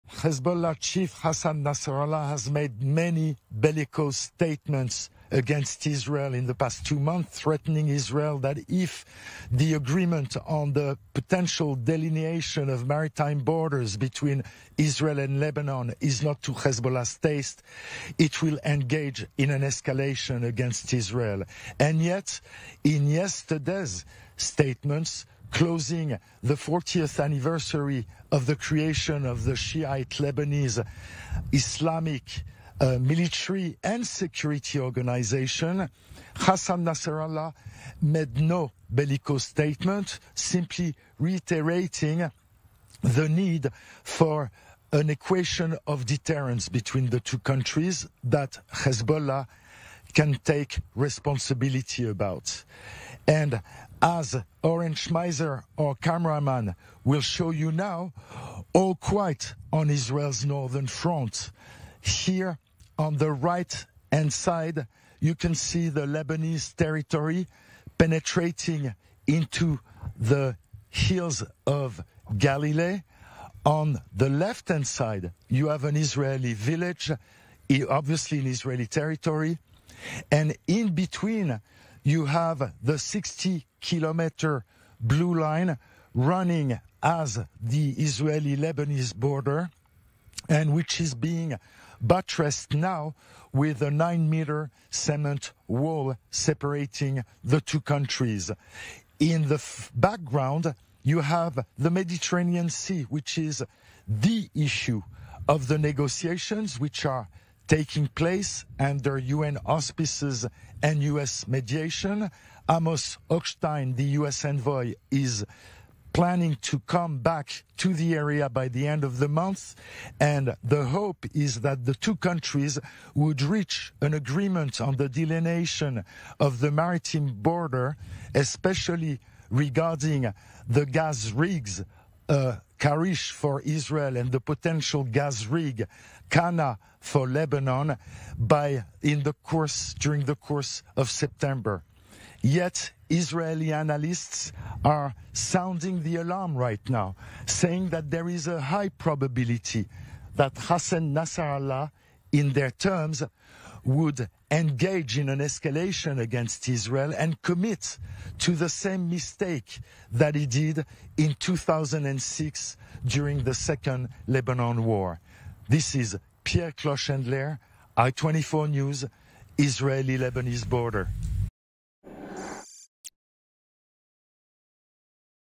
Fragile quiet on northern front, as Israel and Hezbollah exchange warnings
Our correspondent Pierre Klochendler reports from the Israel-Lebanon border where the IDF continues to construct a fortified wall, as Israel and Hezbollah exchange warnings over the ongoing maritime border negotiations.